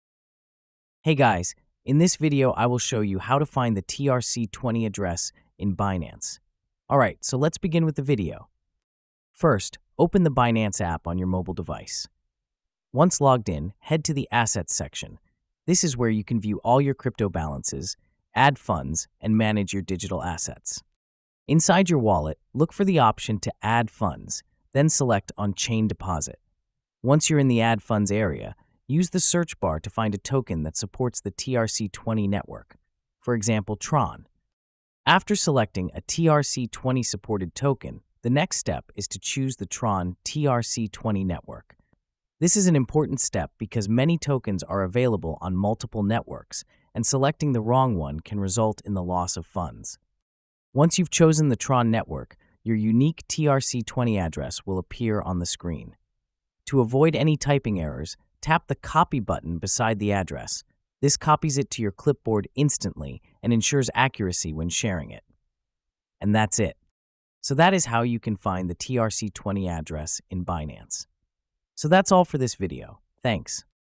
How To Find The TRC20 Address On Binance (2026 Guide)
In this video, you’ll learn how to find your TRC20 address on Binance in 2026. We’ll show you exactly where to locate your USDT (TRC20) deposit address, copy it safely, and use it for secure crypto transfers between wallets or exchanges.